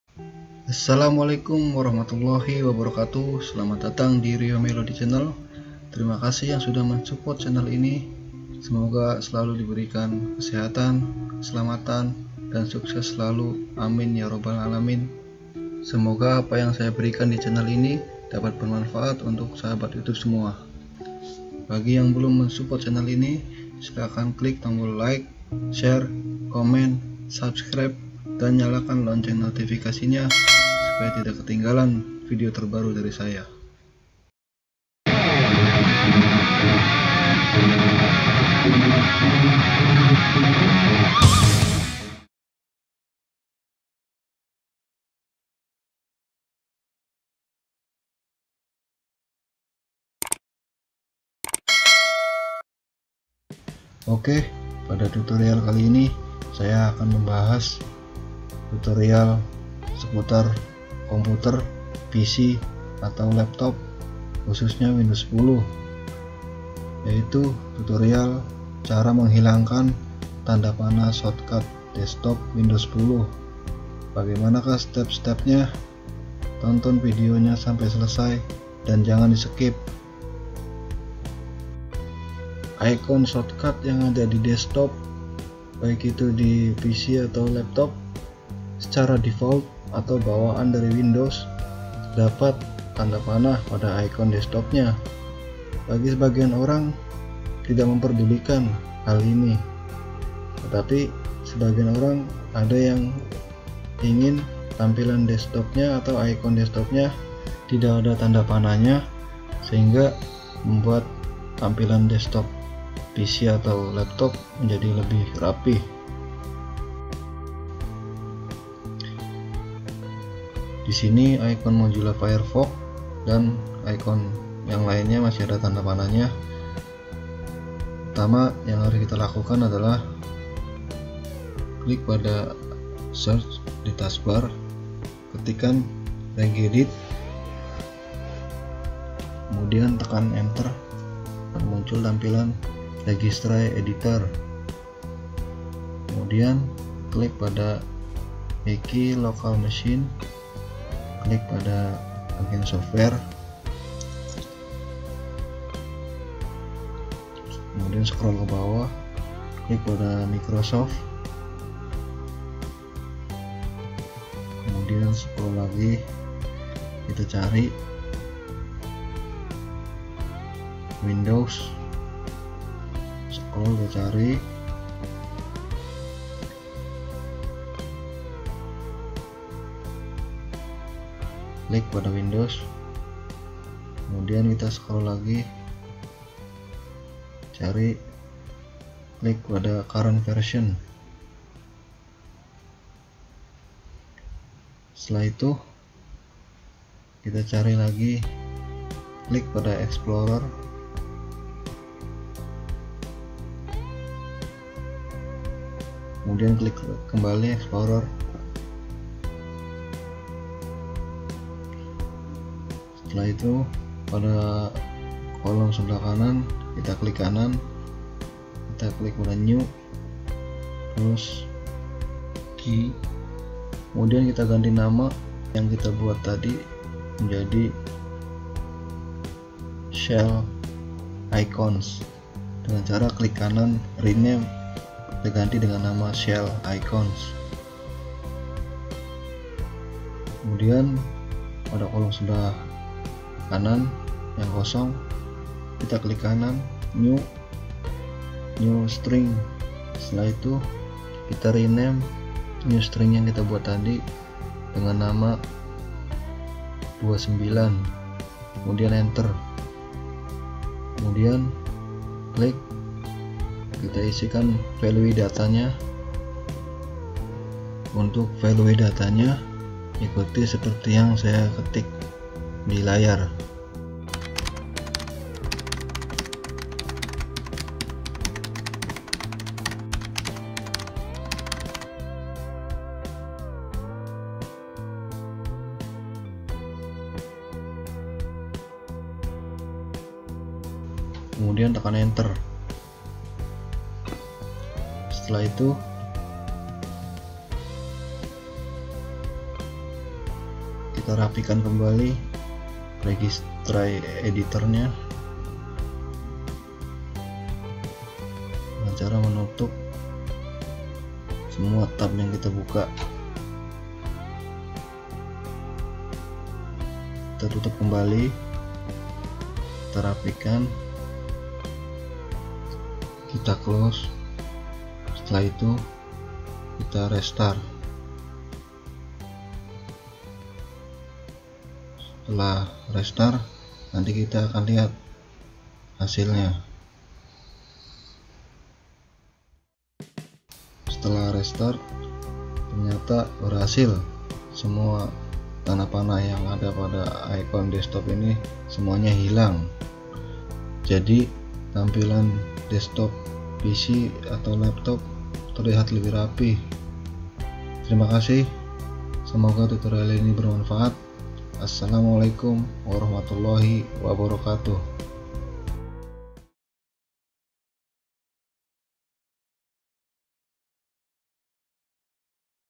CARA MENGHILANGKAN TANDA PANAH SHORTCUT WINDOWS 10
Tutorial cara menghilangkan tanda panah pada icon shortcut desktop di Windows 10.
Cara ini berbeda dengan cara yang digunakan untuk menghilangkan tanda panah pada icon shortcut desktop di Windows 7.
Dengan cara ini menjadikan tampilan desktop PC atau Laptop menjadi lebih rapi.

Terima kasih yang sudah menonton Video ini,